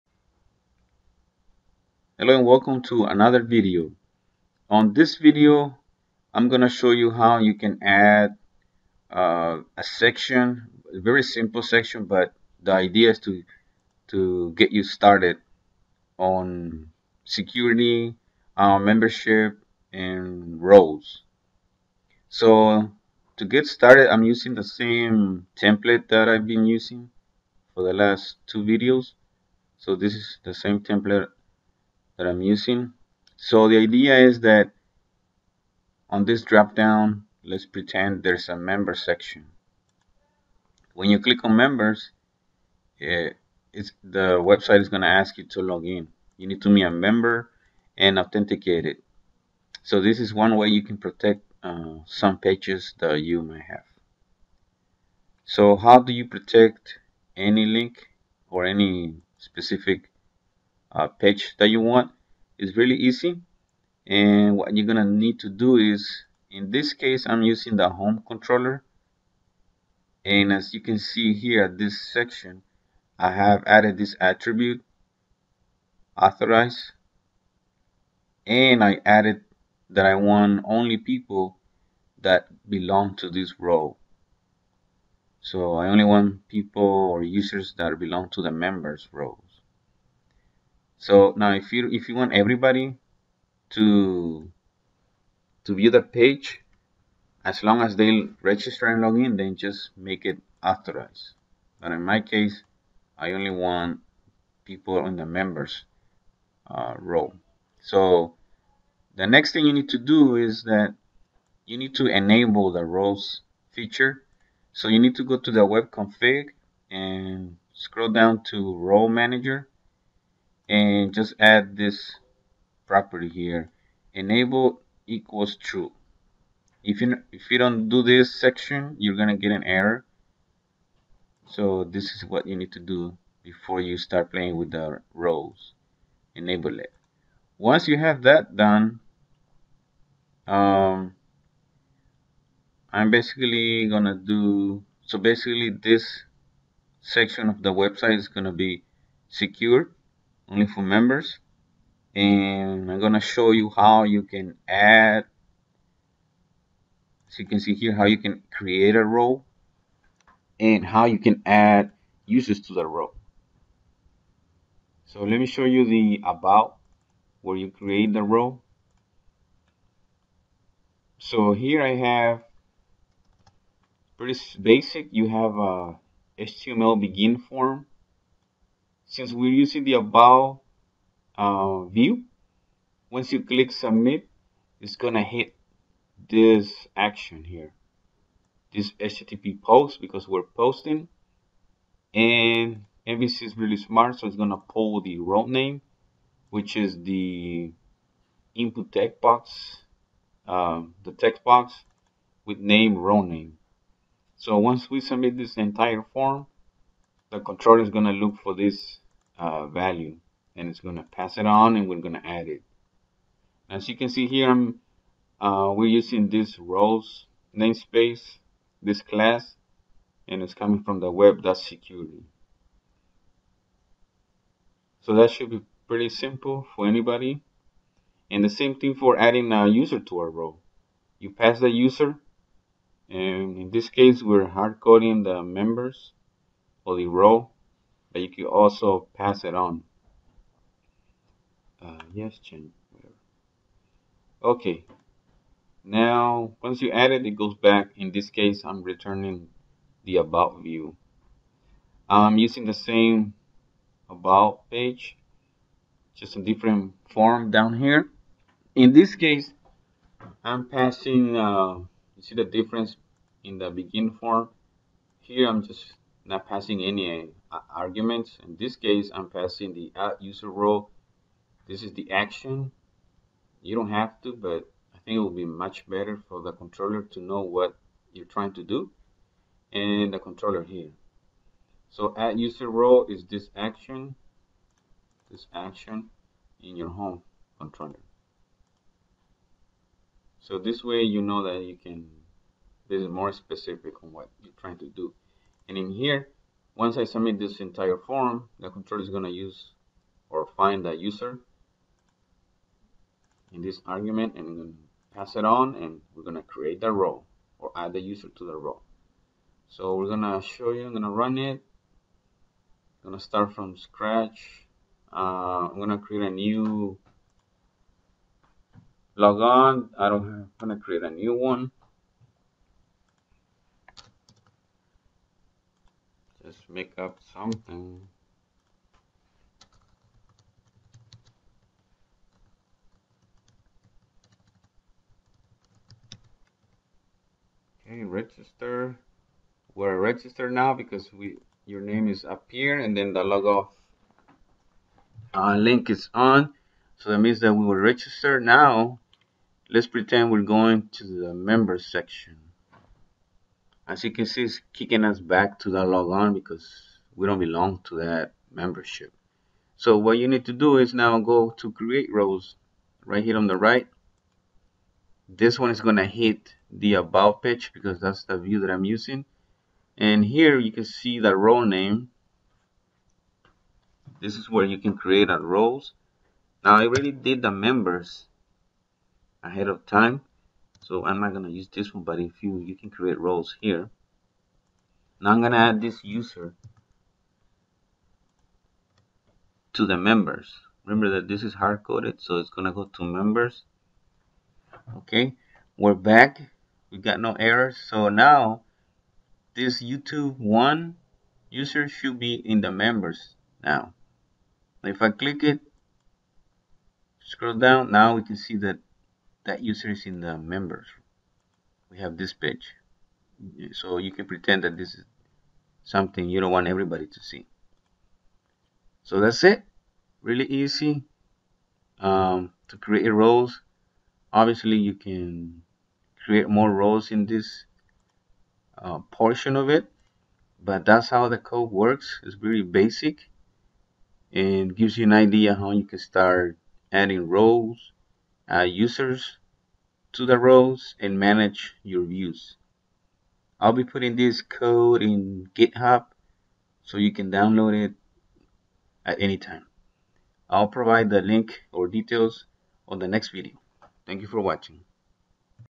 How to secure and manage roles in your MVC web site
Simple video on how to secure your pages on mvc web site. On the next video I will provide details on where to download the code.